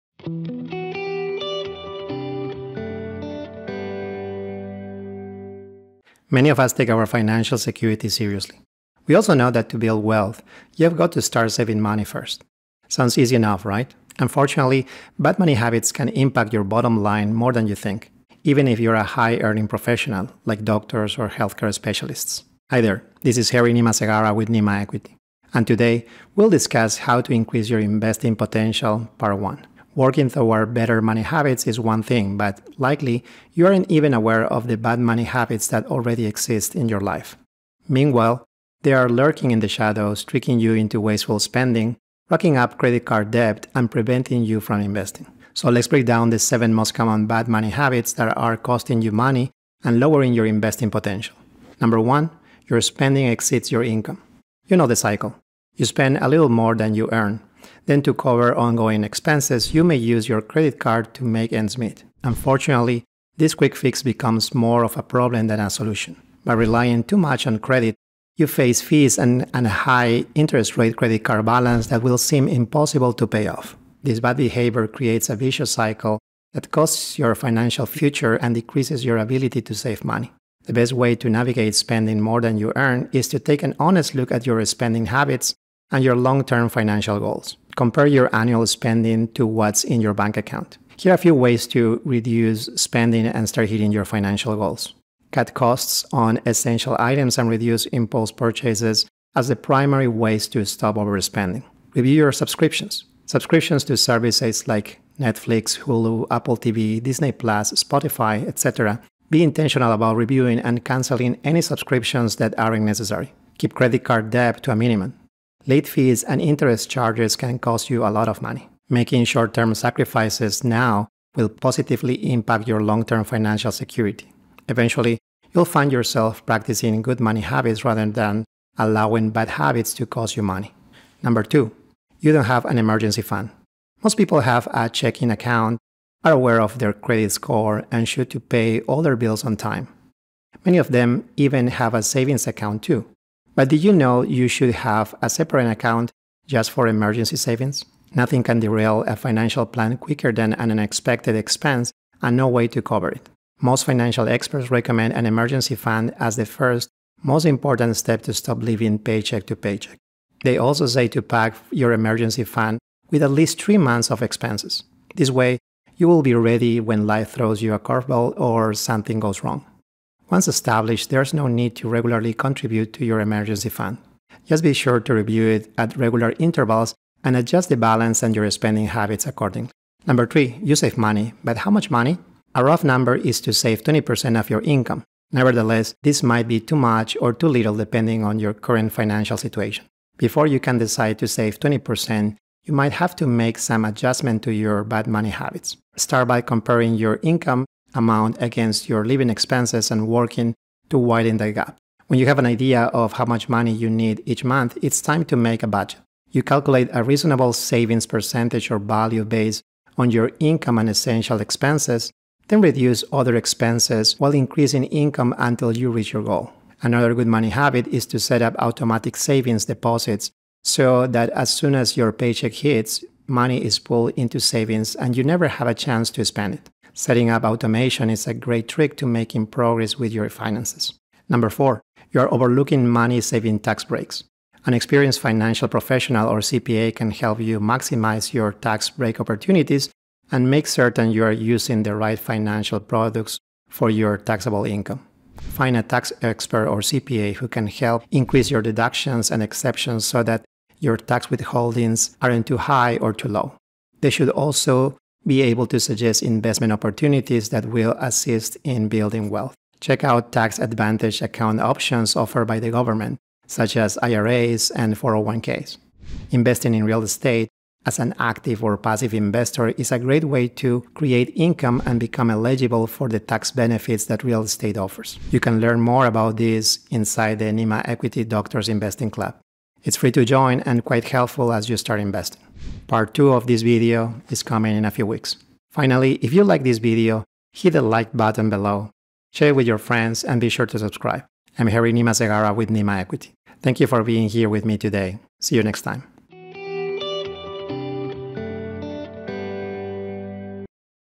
How To Increase Your Investing Potential - Part 1
💰🚀 For many of us, financial security is a top priority and we understand that building wealth starts with saving money. It seems straightforward, doesn't it? However, the truth is that bad money habits can have a more significant impact on your financial health than you might realize, even if you're a high-earning professional, like doctors or healthcare specialists.

Today we will explore ways to boost your investment potential. While striving for better money habits is great, you might not even be aware of the detrimental money habits that have silently crept into your life. These habits hide in the shadows, leading you into wasteful spending, piling up credit card debt, and hindering your investment journey.💰🏡

Let's dive in as we uncover the seven most common bad money habits that could be costing you money and limiting your investment potential. This is Part 1! 📈💡

To learn more about investment opportunities, join the Doctors Investing Club